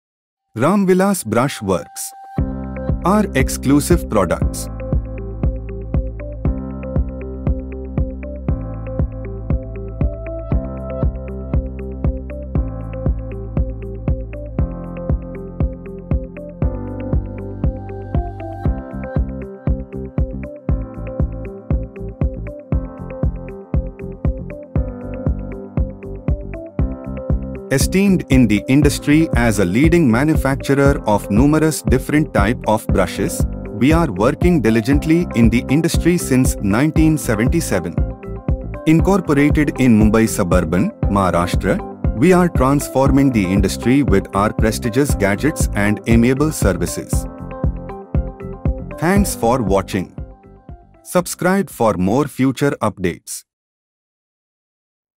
Ramvilas Brush Works || Company Store - Aajjo.com
Ramvilas Brush Works is a well-renowned organization, providing quality services in the industry since 1977. With an adept knowledge of the industry, they are appreciated for their quality-assured product line and fruitful services.

It is a Mumbai Suburban, India-based organization, working amicably in the industry to ease clients’ business & reduce labor with applicable accessories. With a mass number of satisfied consumers, they are acknowledged as a leading manufacturer, seller, and trader of various different types of Brushes.

Some of their exclusive products, featured in the video are:
• Strow Tube Cleaning Brush
• Artificial Turff Brushing Brush
• Cylindrical Roller Brush
• Fly Ash Brick Making Machine Brush
• Machine Cleaning Brush
• Makita Nylon Brush
• Road Broomer Ring Brush
• Road Sweeper Brush

Or download our AAJJO app now.